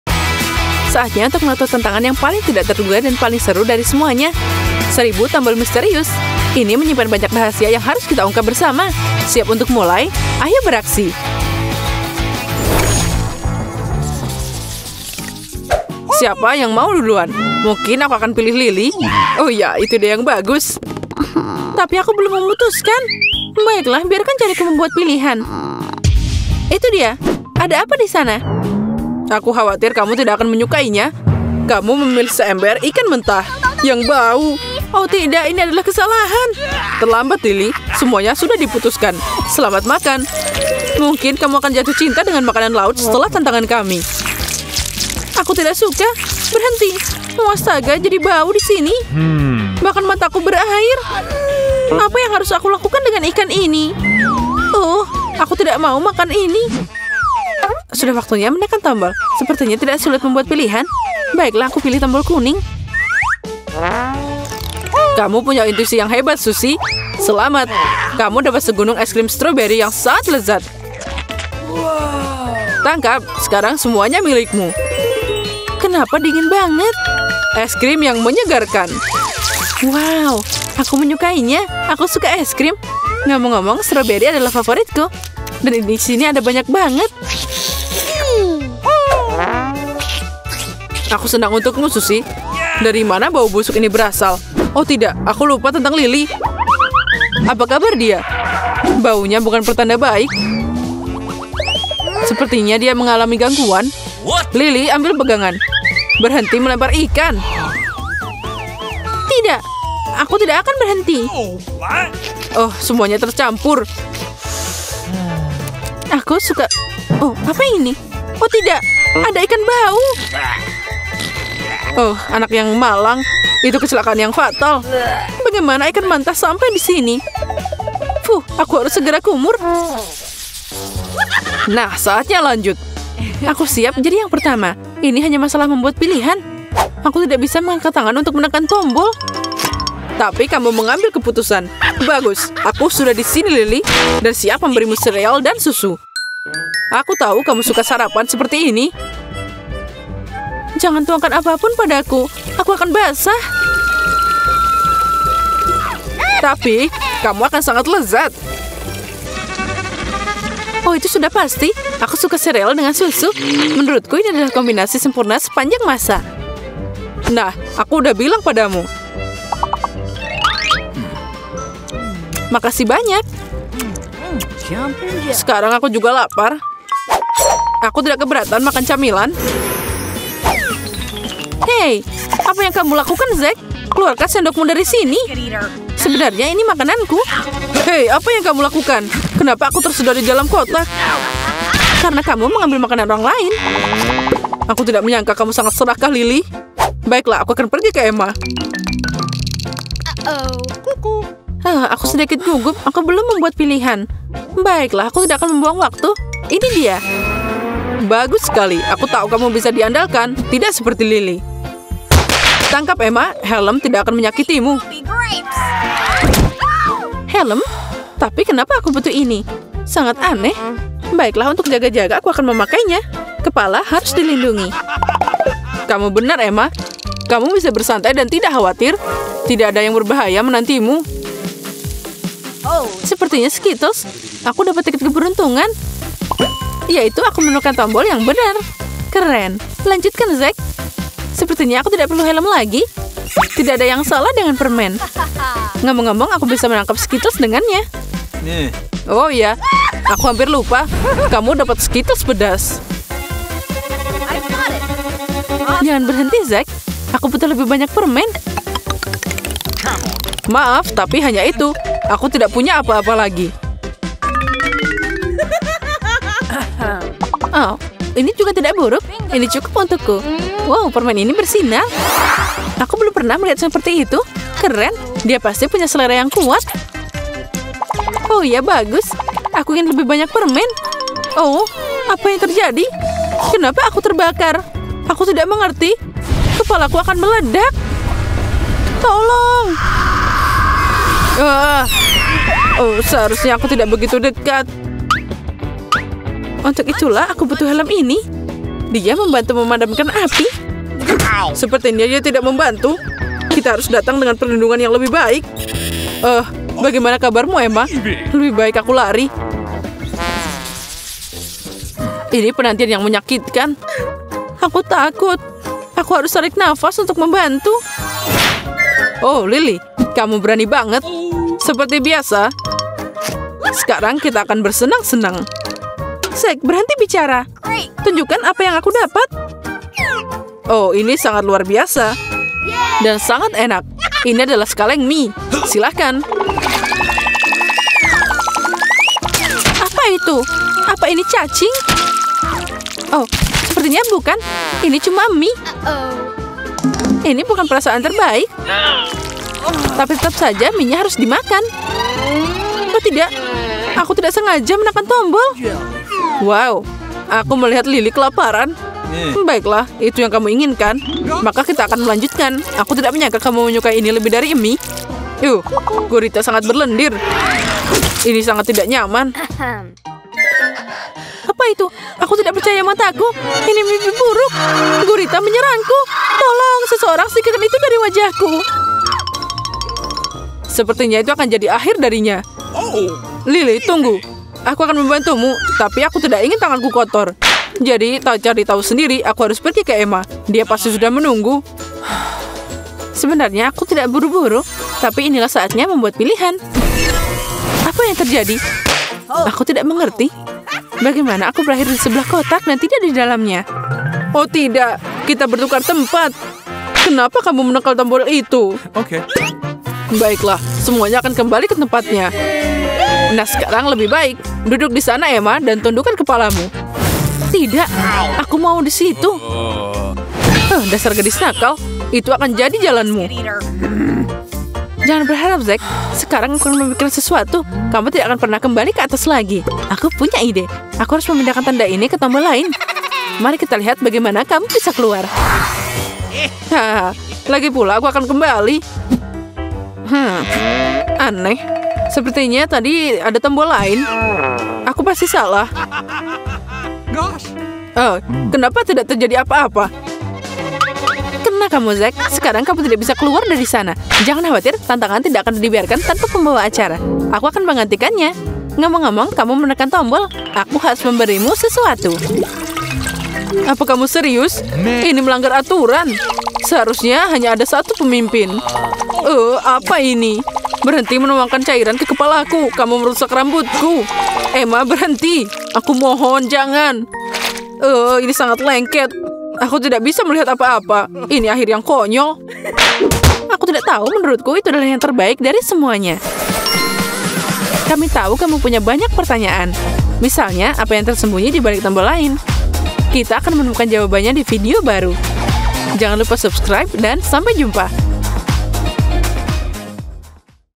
Tantangan 1000 Tombol Misteri #4 | Cuma 1 yang Ngelolosin Kamu Multi DO Fun Challenge
Saatnya untuk tantangan yang paling tidak terduga dan paling menyenangkan dari semua yang mungkin - Seribu Tombol Misterius! Itu menyimpan banyak rahasia yang harus kita ungkap bersama! Siap untuk mulai? Ayo pergi!

Share video ini ke teman-temanmu untuk menantang kebolehan mereka saat makan yang manis-manis!